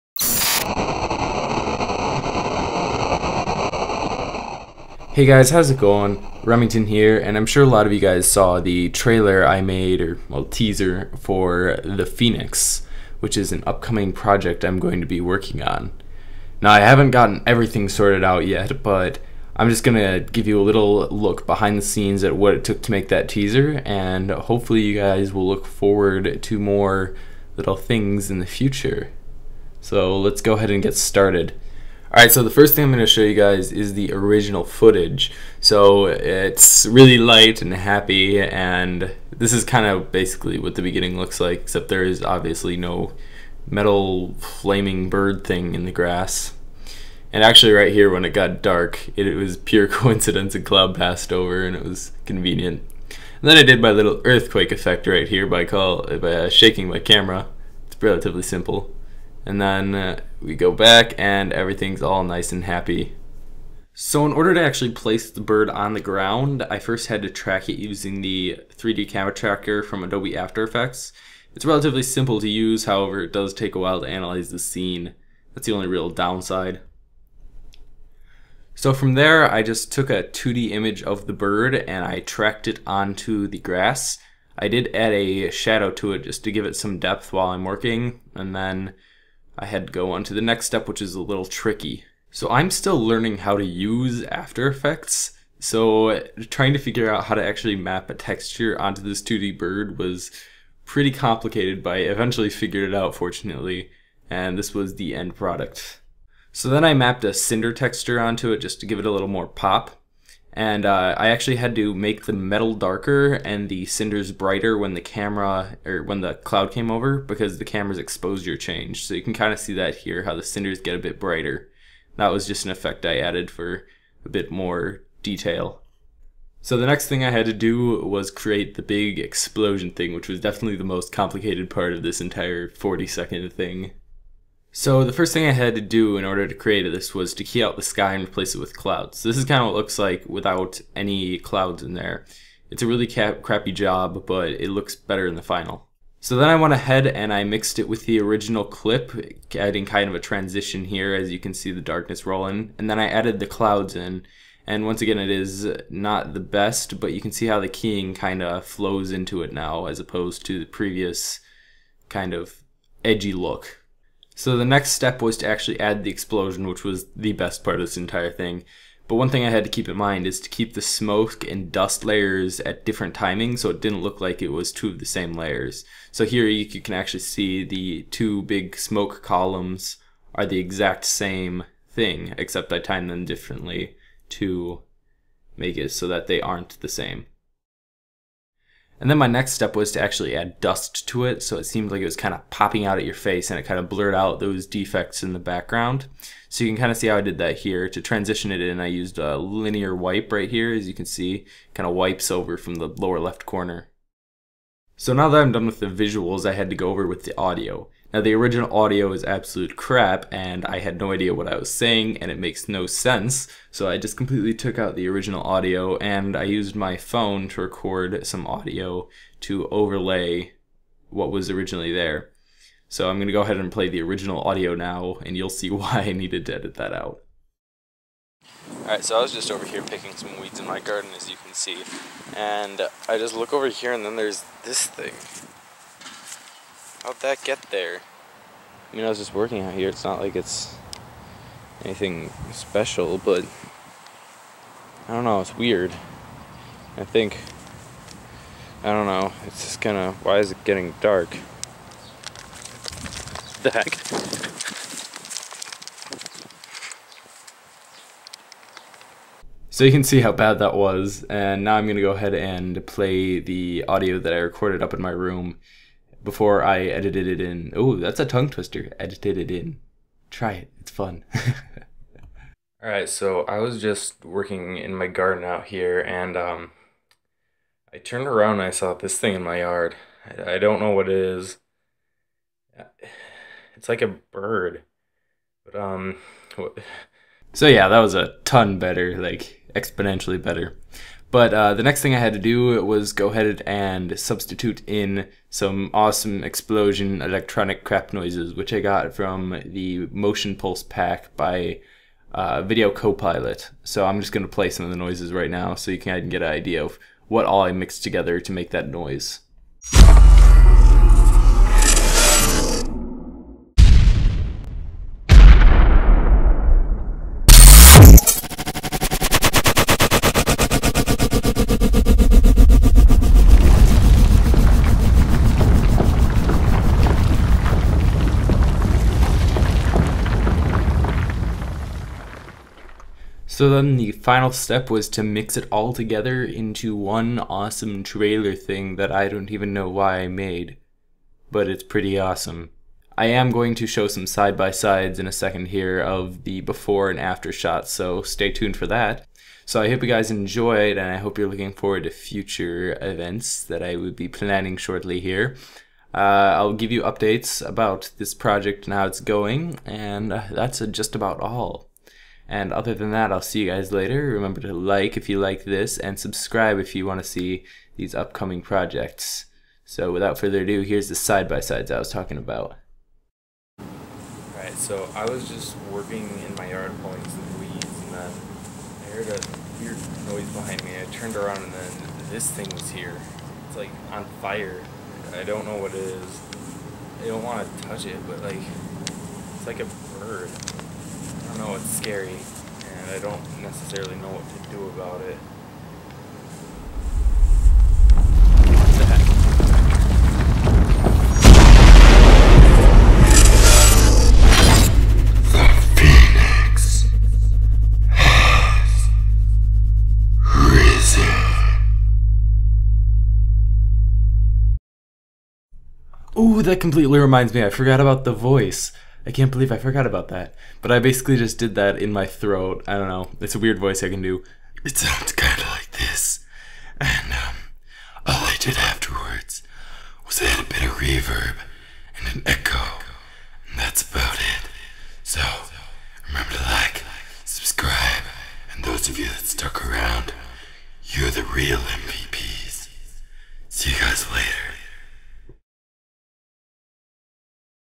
The Phoenix Project [Behind the Scenes Pt1]
Just a little look at what I'm planning to do with this project and the work I put into the different effects and audio. I hope you guys enjoy!

Softwares used:
Adobe After Effects - Visual Effects and Intro
Sony Vegas Pro 12 - Audio/Video Editing
Audacity - Noise removal & recording

Graphics Packages Used:
Motion Pulse - Video Copilot
Action Essentials 2 -Video Copilot